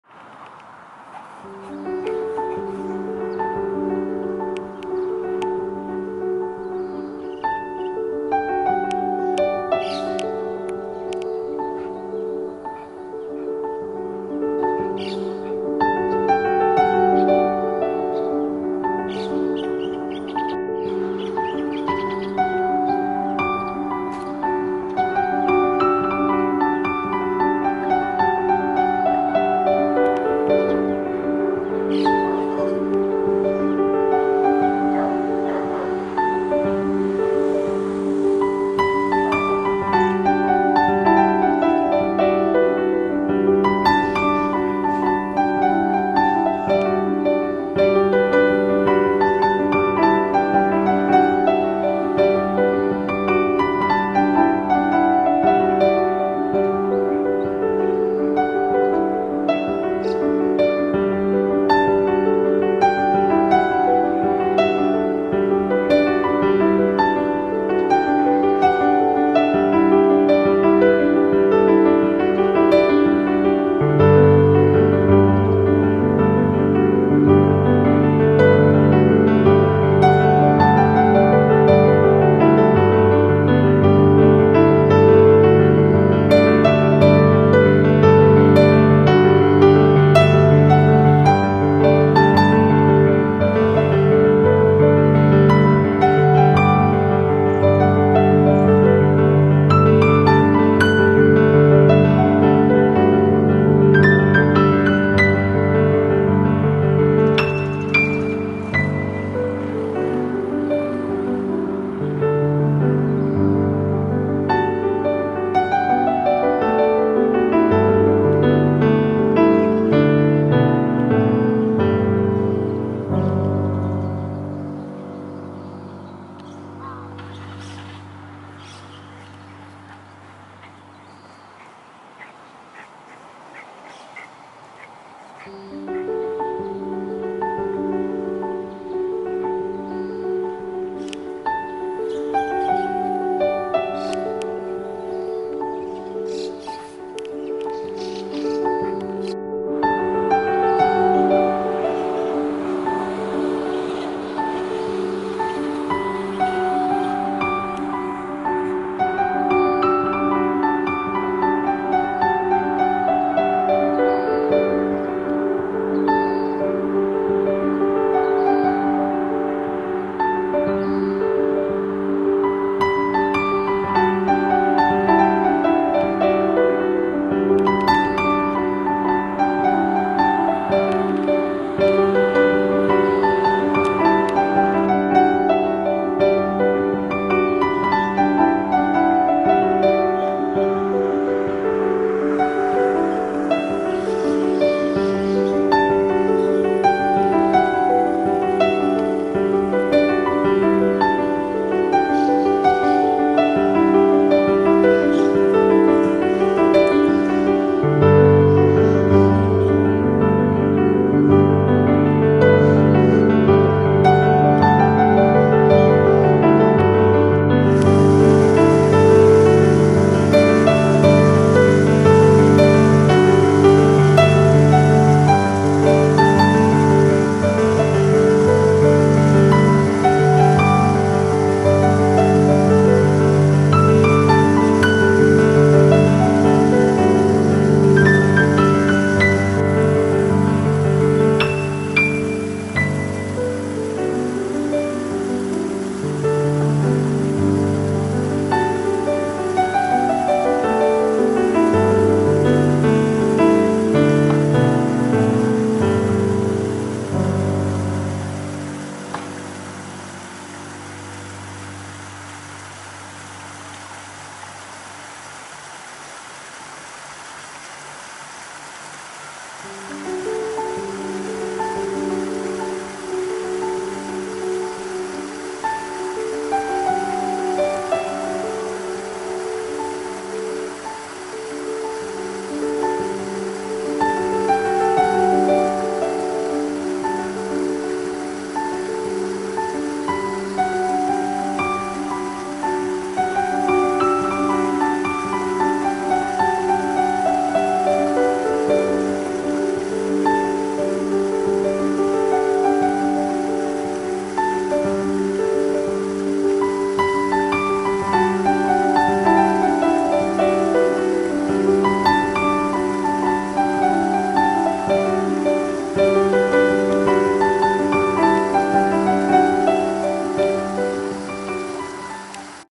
二子玉川Bird sanctuary 野鳥観察図鑑 雉鳩朝から夕まで働く 2023年5月1日
二子玉川Bird sanctuary
野鳥観察図鑑
雉鳩　朝から夕まで働く
2023年5月1日